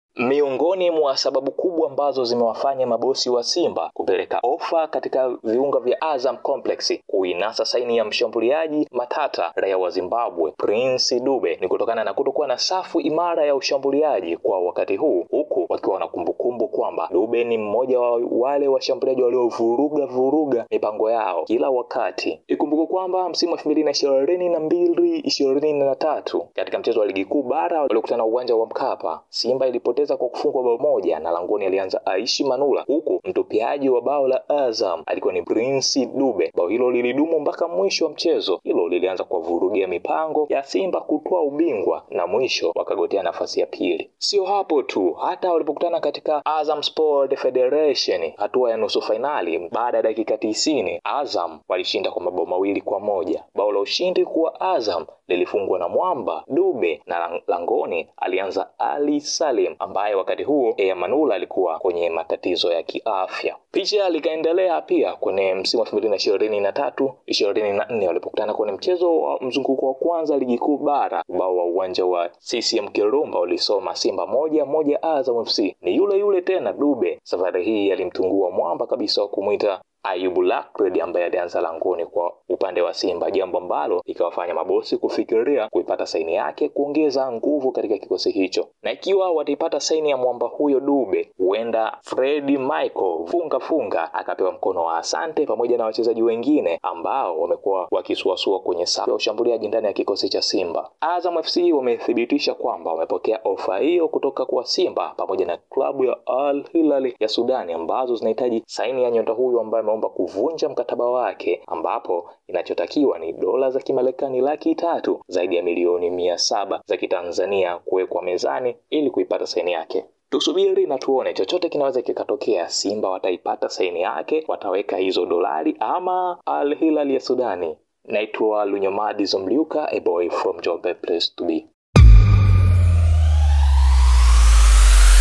ANAONDOKA NA MTU PRINCE DUBE NDANI YA SIMBA
ANAONDOKA na mtu Prince Dube ndani ya kikosi cha Simba ambacho kinaisaka saini ya nyota huyo aliyewapa tabu mara nyingi walipokutana kwenye mechi za ushindani ndani ya uwanja.